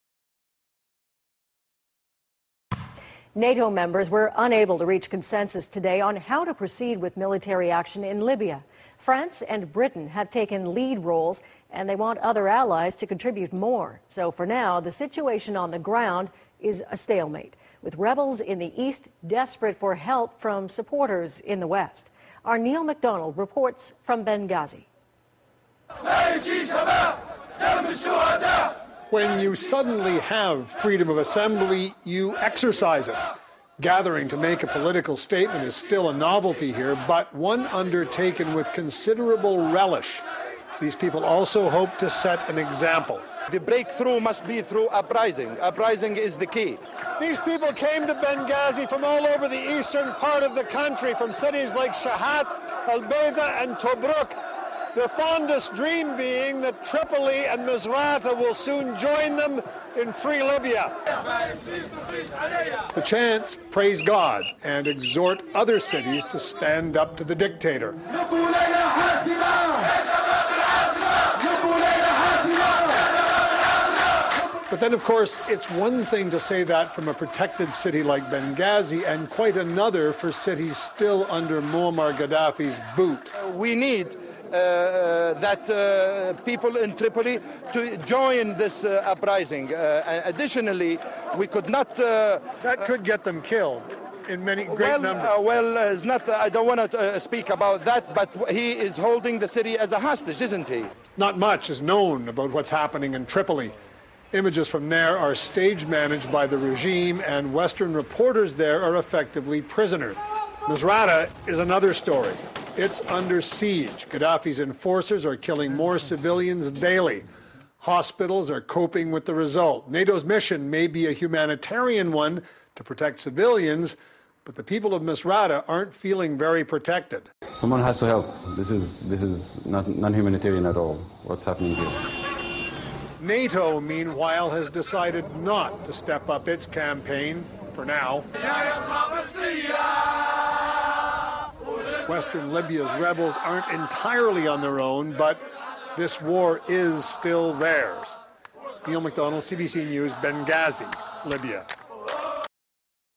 Libya: a country divided
If nothing else about the civil war raging in Libya, you have to give the rebels this:  once they got a taste of civil rights, they not only exercised them, they're taking their civic duties responsibly as well.